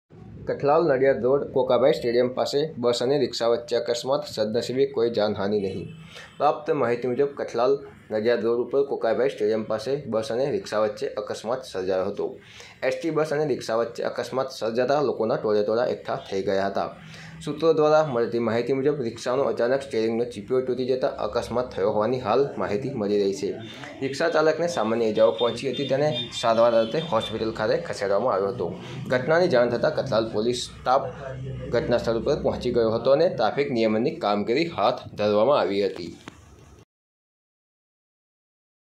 કઠલાલ એક્સિડન્ટ/kathlal accident
કઠલાલ નડિયાદ રોડ ચાર રસ્તા પાસે રીક્ષા અને બસ વચ્ચે અકસ્માત